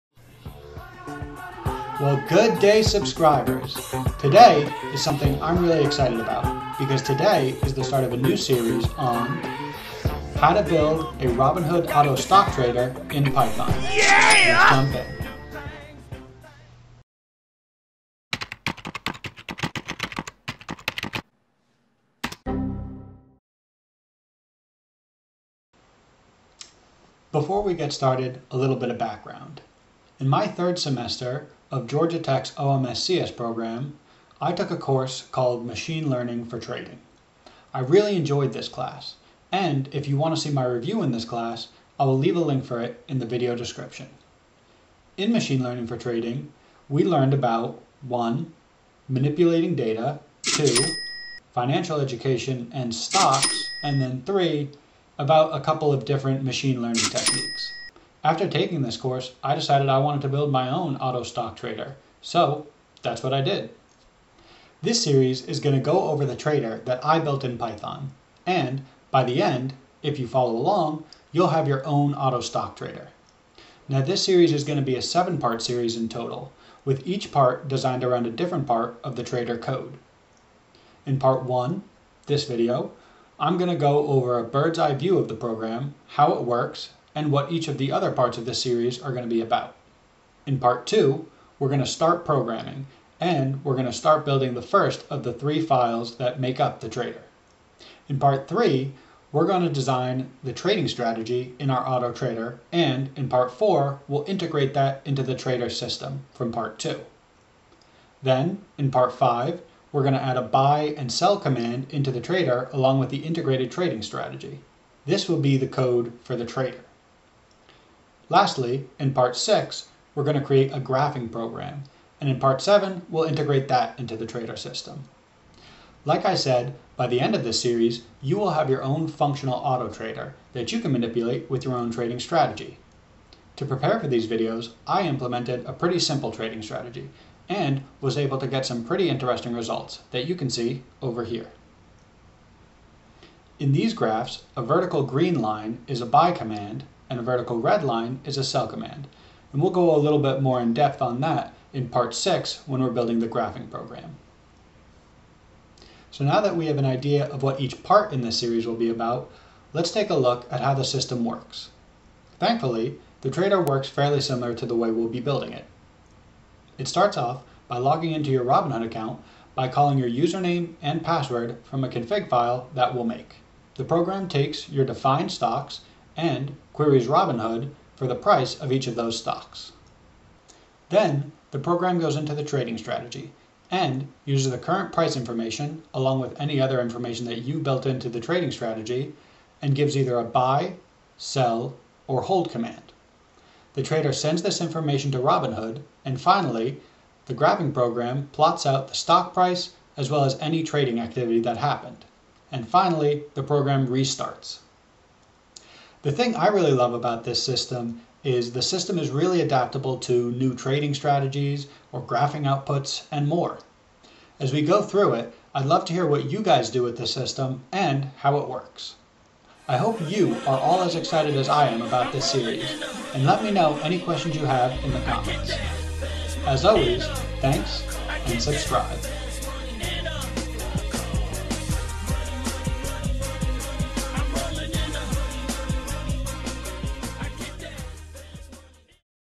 Robinhood trading bot - How to Build a Robinhood auto-stock trader in Python (part 1)
I have always wanted to build my own trading bot to trade stocks for me and in this video, I share with you how I was able to build a trading bot to trade on Robinhood!

This is the start of a new series on how to build a Robinhood trading bot in Python. In part 1, we will go over the outline of the series, how we are going to build our own trading bot, and what will be covered in each part.

The trading bot will connect with Robinhood and trade on the live stock market based on a trading strategy we will design in a future video. Follow along and by the end, you will have your own fully functioning trading bot!

Thank you to my brother for editing the video!